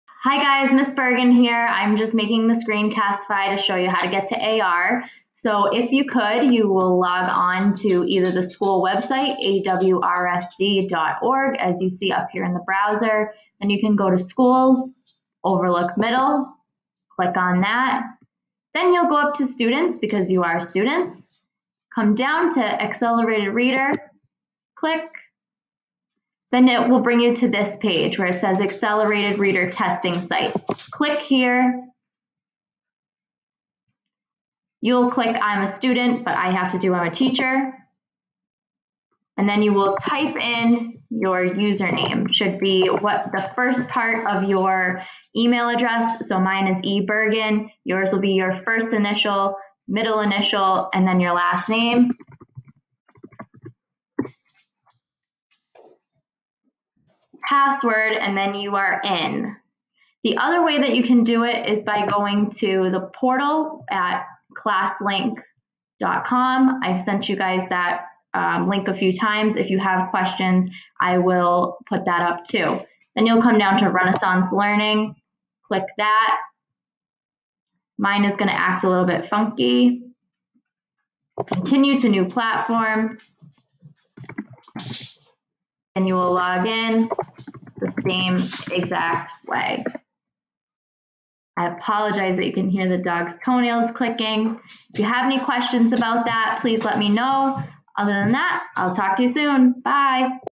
How to access Accelerated Reader (AR) from home.
Hey Overlook Middle! Here is a "how-to" video on accessing the AR website from home.